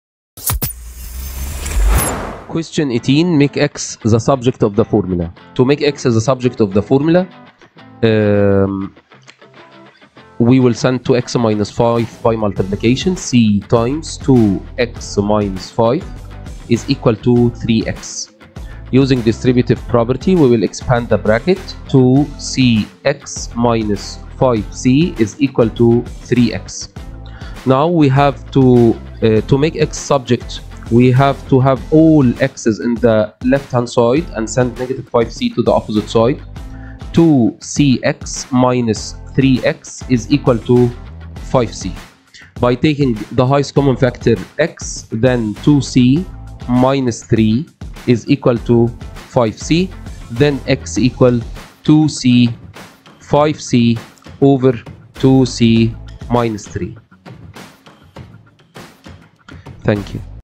0580 22 m j 23 Q18
All of the IGCSE Mathematics
step-by-step solution for the latest IGCSE exam
In this playlist you should be able to see the following:
Cambridge IGCSE Mathematics 0580/22/M/J/23 discussion and solution
Mathematics 0580 | Paper 2 Variant 2 | May / June 2023 || Full Length
0580/22/m/j/23